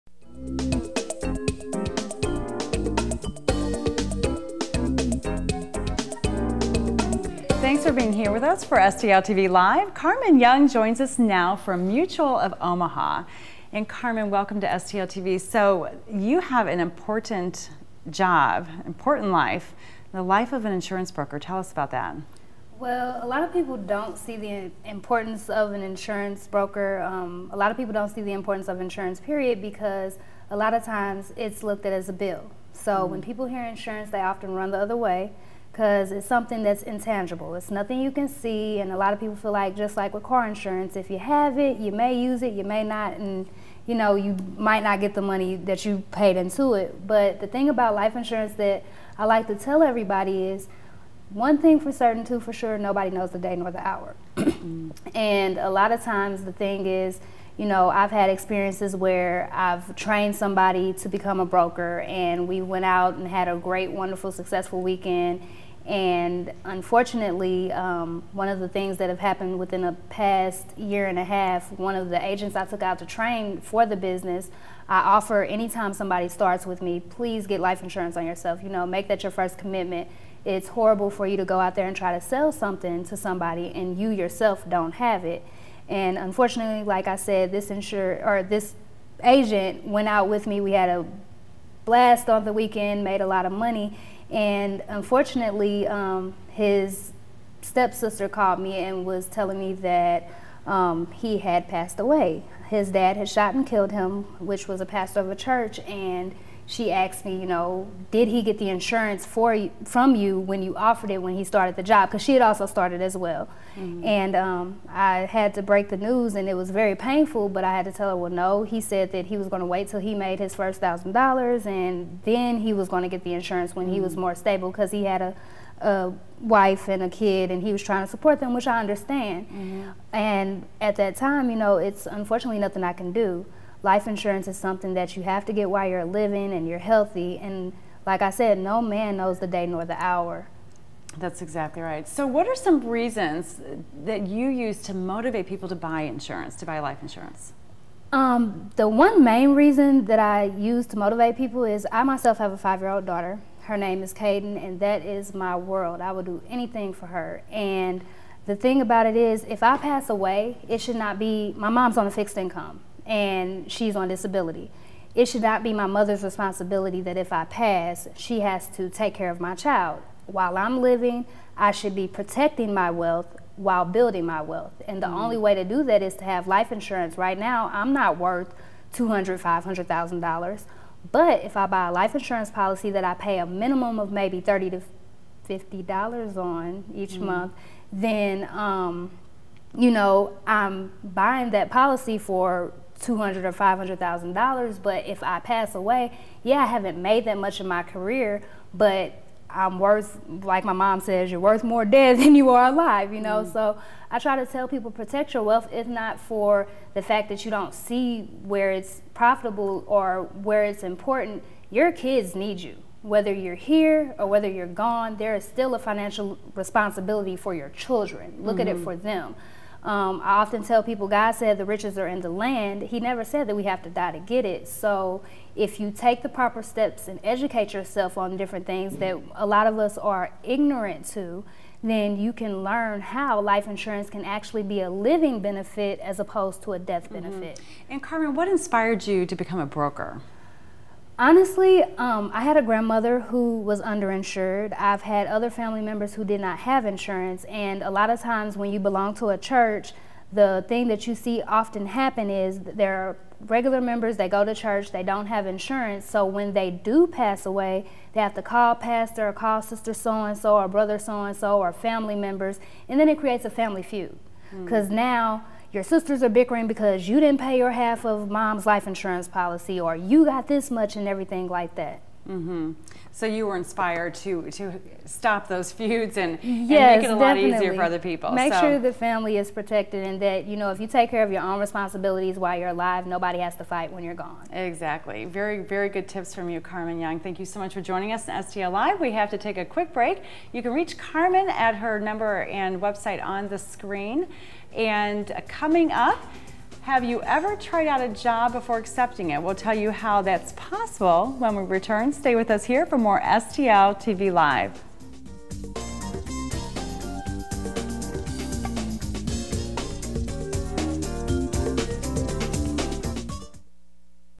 STL TV LIVE - Mutual of Omaha Community Outreach Program
STL TV LIVE - Community Outreach Program
Carmen Young is now recruiting. Her Community Outreach Program is helping to break the chains of poverty by offering jobs to the unemployed as well as the under employed. If you would like to talk with Carmen, you can contact her at 482-1001

Community Outreach Program
Carmen C. Young